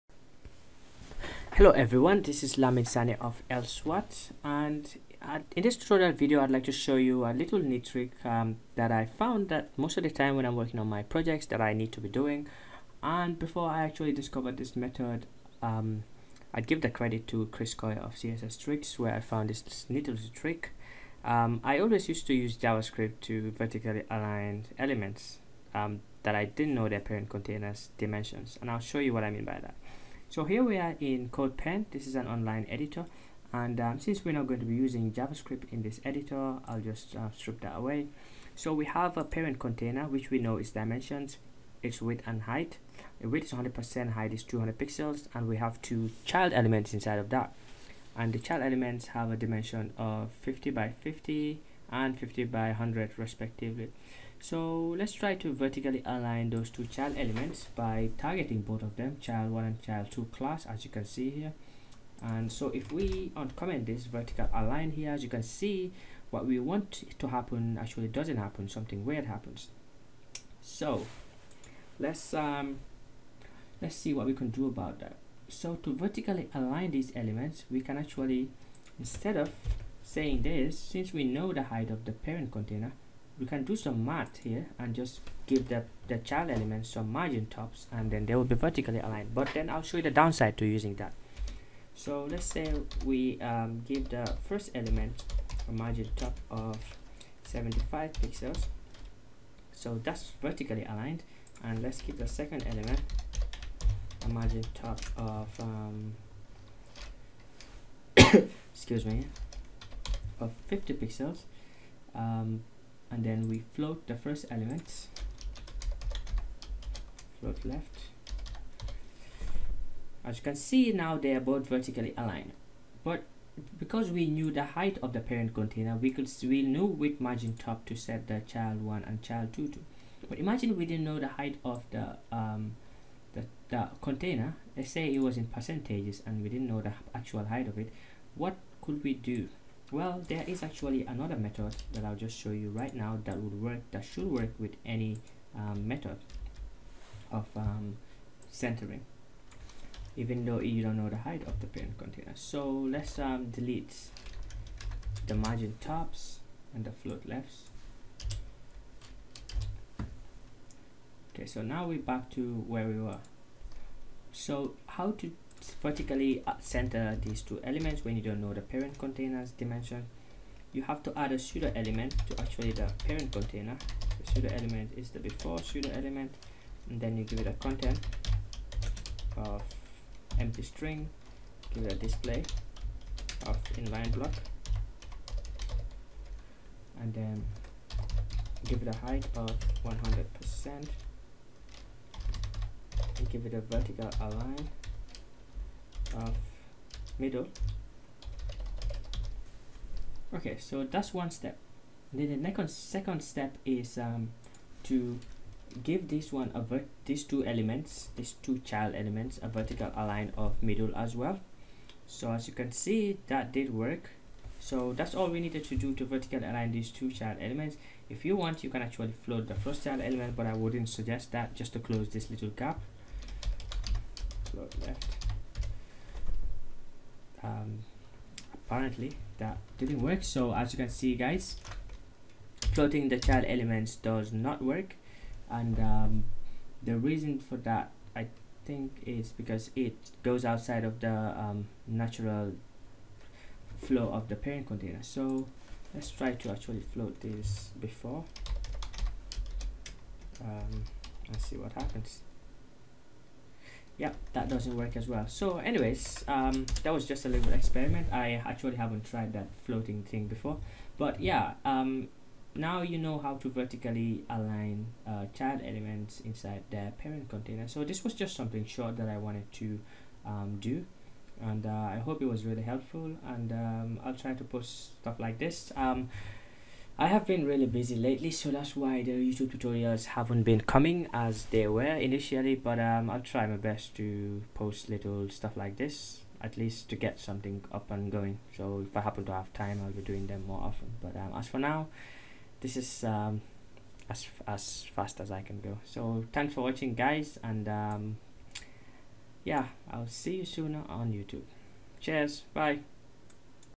How to Vertically Align Text in a Div
In this tutorial video, we shall learn how to align some text or element inside another div or its parent container.